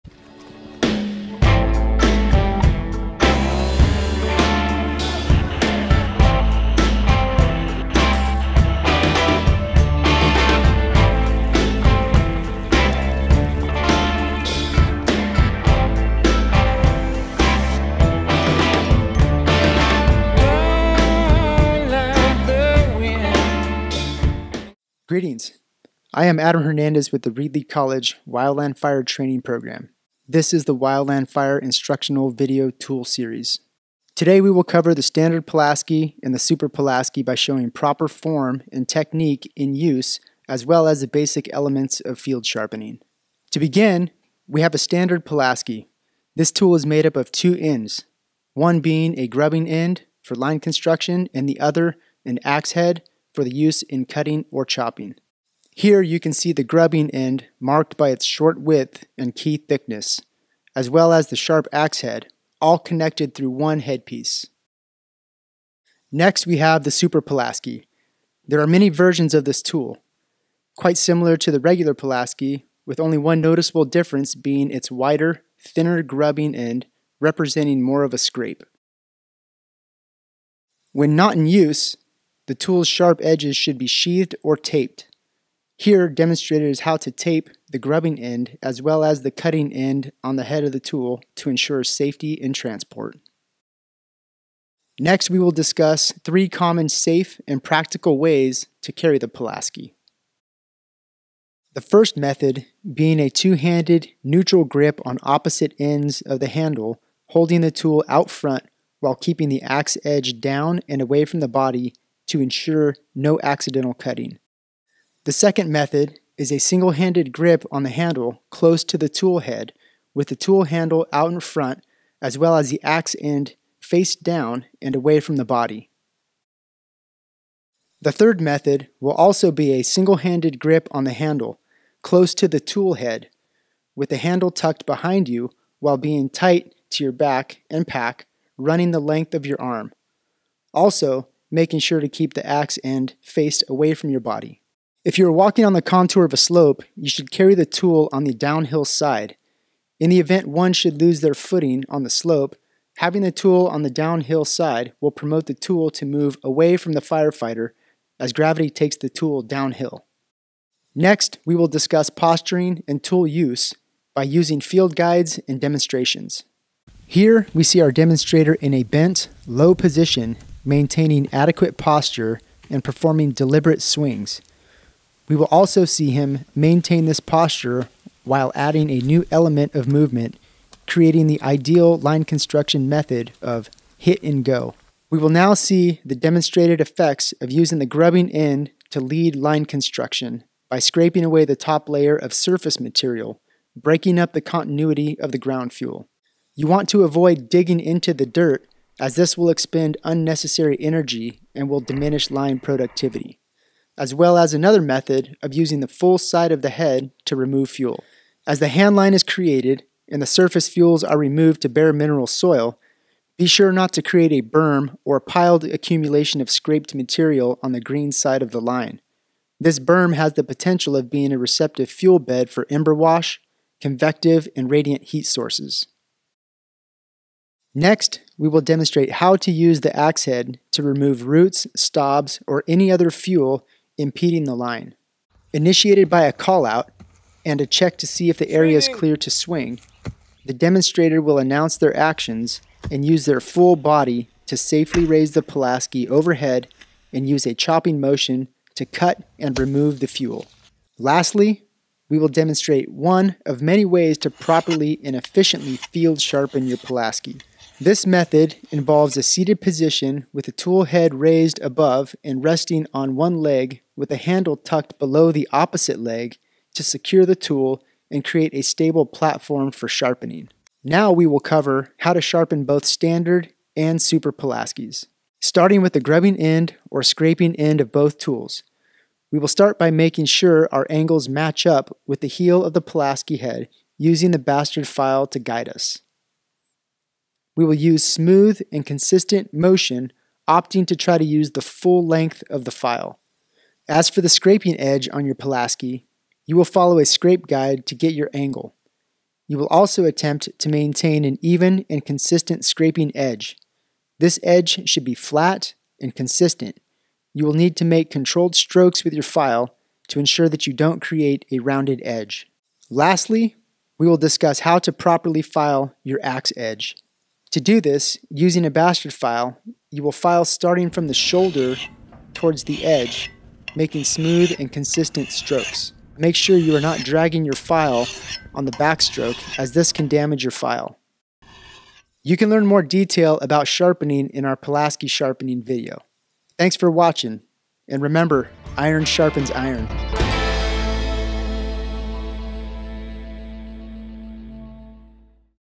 PULASKI VIDEO TOOL SERIES
In this instructional video we cover the Pulaski and the Super Pulaski. We will discuss proper form and technique in use as well as the basic elements of sharpening.

REEDLEY COLLEGE WILDLAND FIRE TRAINING PROGRAM

Thanks to the Kern Valley Hotshots for their Scrapers Field Book and Right Cards